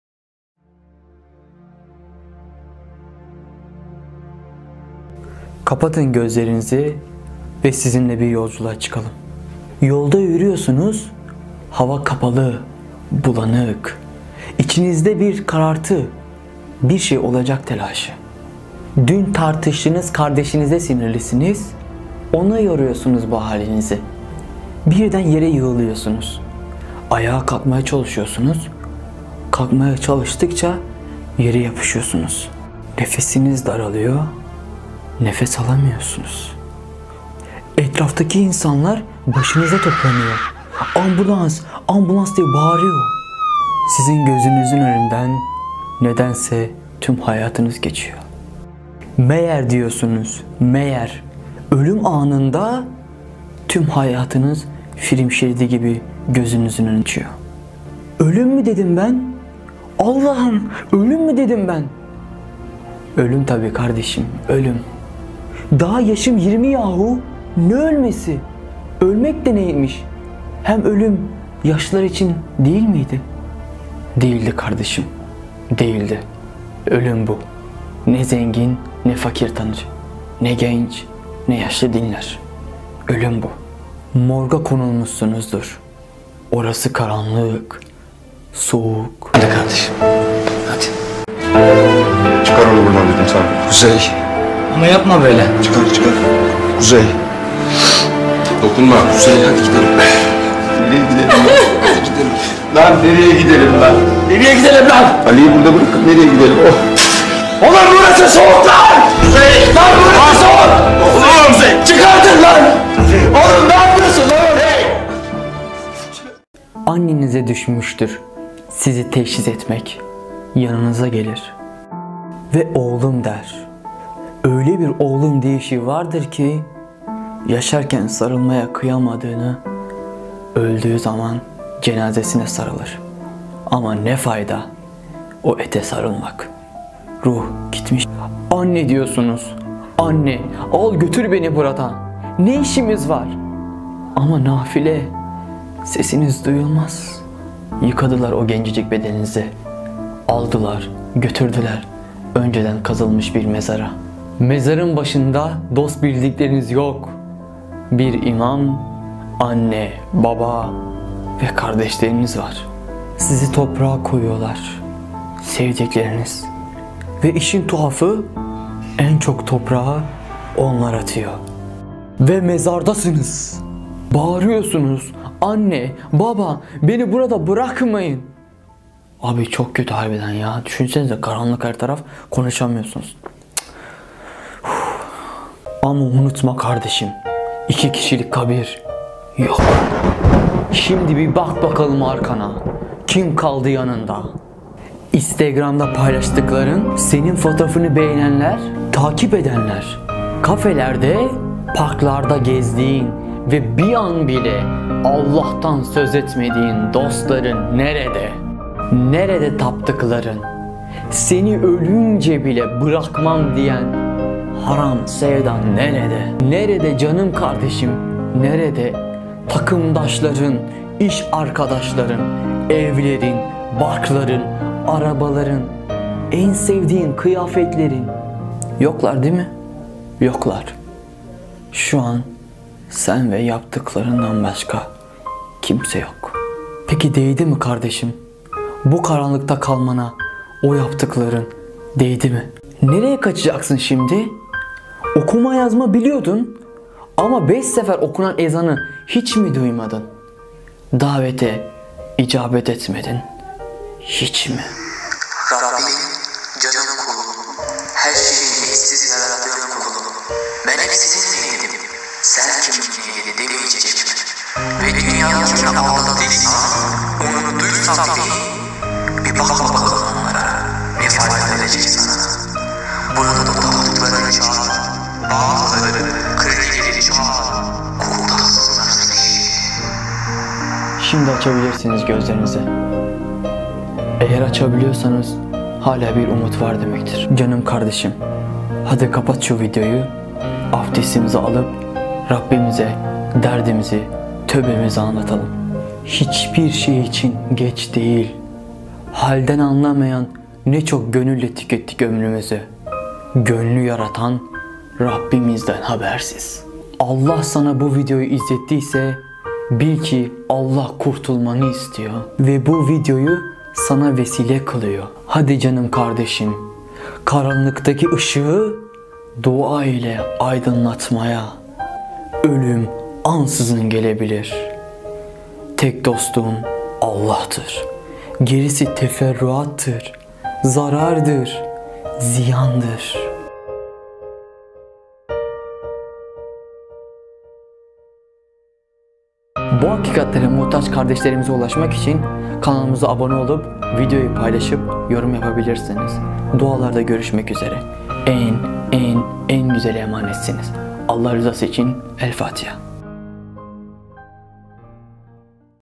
İzlemesi Zor Bir Video Oldu - süleyman yılmaz
GELİN SİZE BİRKAÇ DAKİKADA ÖLÜM ANINI ANLATALIM DEDİK BU VİDEO ORTAYA ÇIKTI HAKKINIZI HELAL EDİN.

“SEBEP OLAN, YAPAN GİBİDİR.”

DİĞER DİKKAT ÇEKİCİ VİDEOLAR :)

EN GÜZEL'E EMANETSİNİZ..
HAKKINIZI HELAL EDİN.